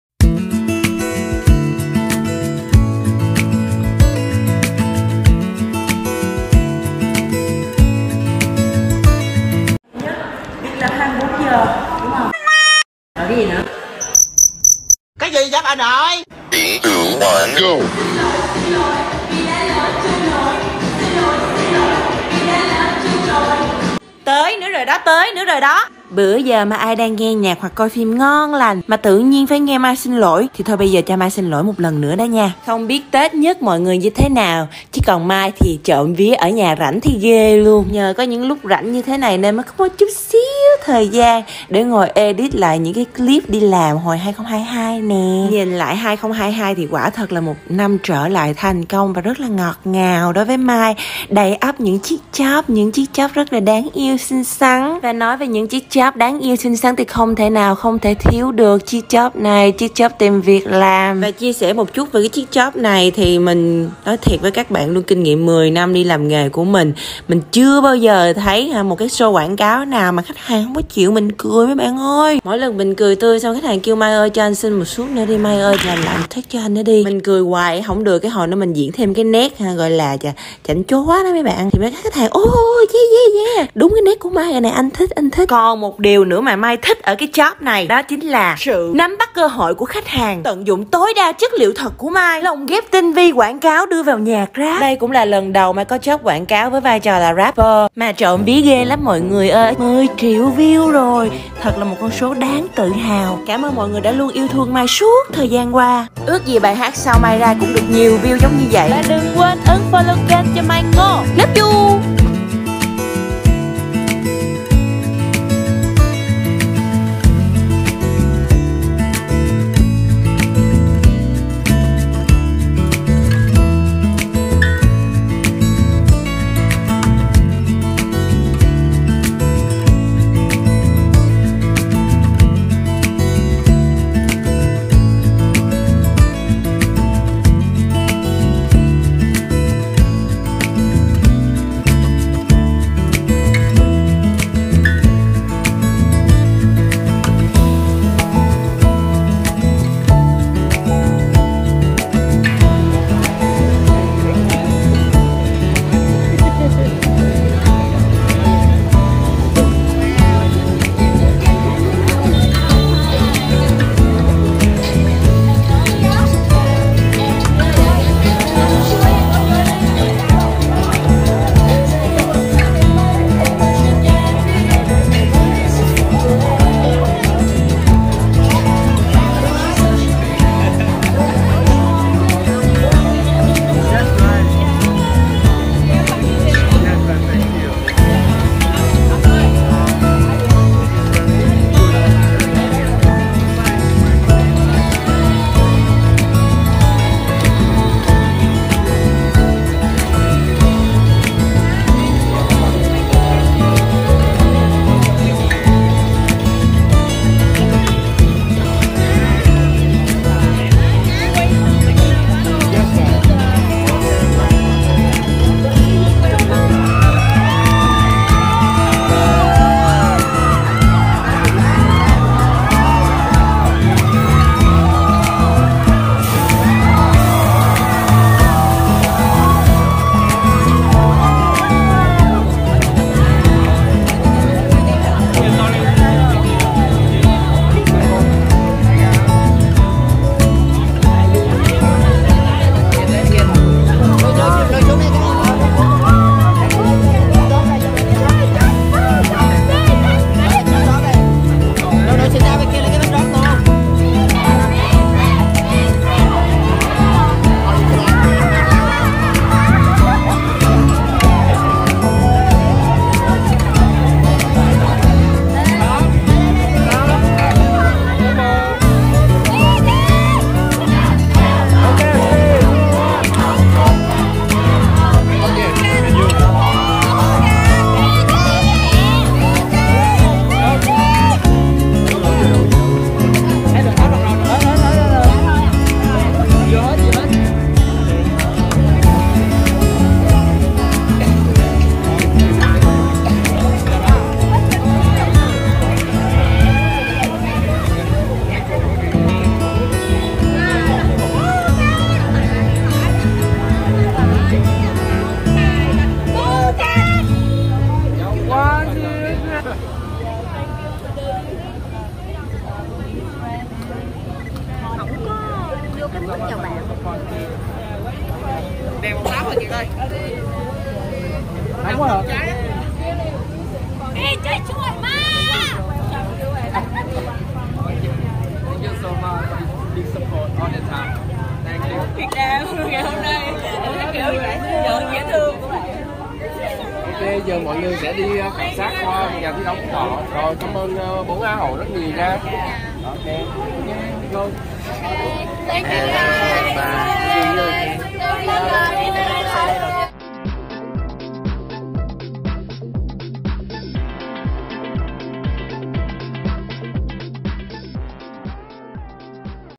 MAI NGÔ ngỏ ý muốn thi Quốc Tế ngay khi MR NAWAT đến VIETNAM, FAN đề xuất MISS GRAND INTERNATIONAL
MAI NGÔ ngỏ ý muốn thi Quốc Tế ngay khi MR NAWAT đến VIETNAM, khoe thành tích xuất sắc trên MXH